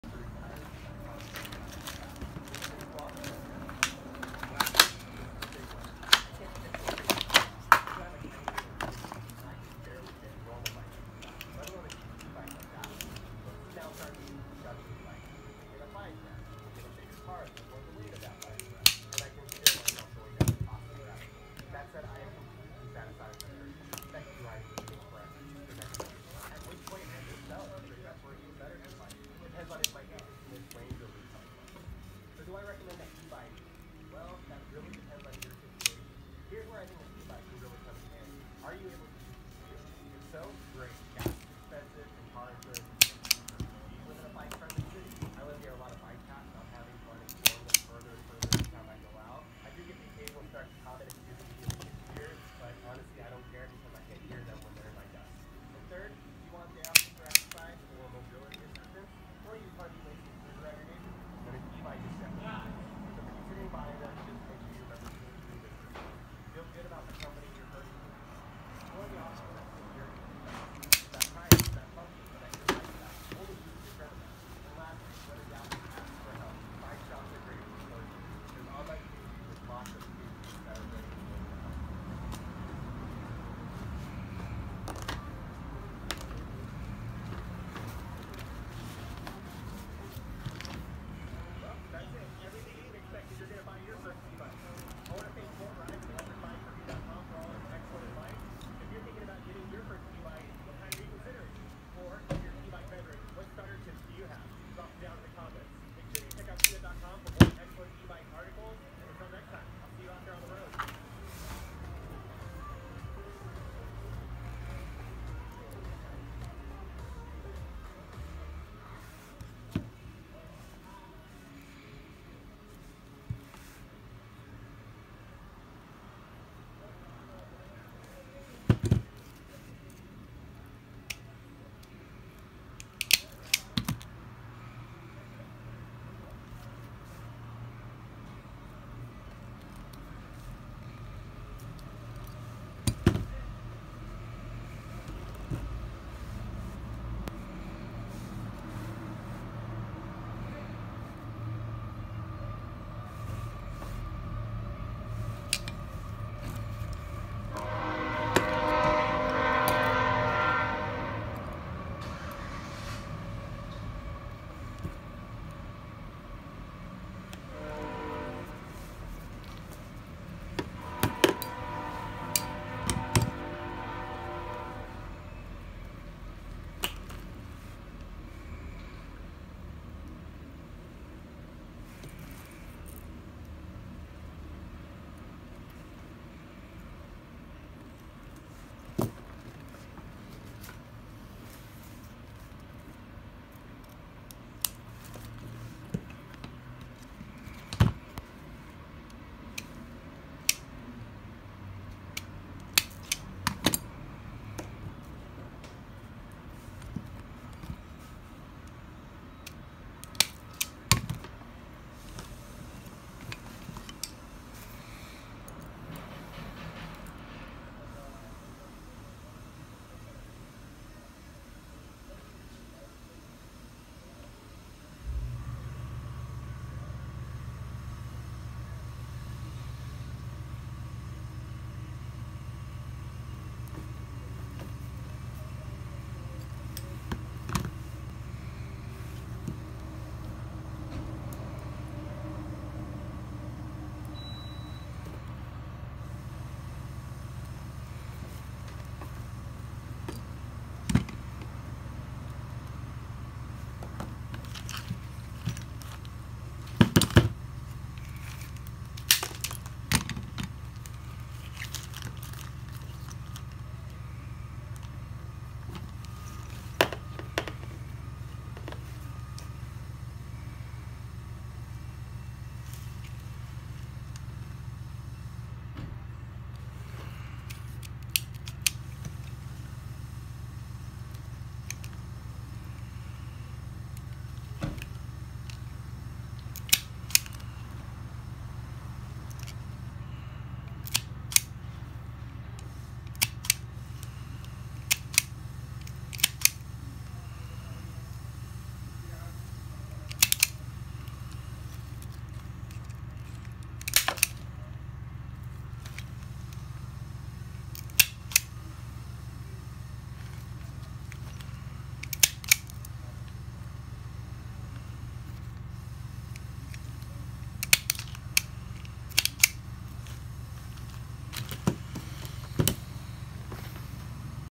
Pittsburgh 6-1/2 wire stripper item # 63143
Testing. The wires are o2 are harder to strip. Most auto strippers don’t do very good. But I was about to get the job done. I’m making a jumper harness so I can test timing on certain cars like Honda and Toyota.

This stripper is as good as the original as sold on tv one. I use to have one but lost it. And I wanted to test these out cause it was cheaper then most on amazon and harbor freight has warranty on these.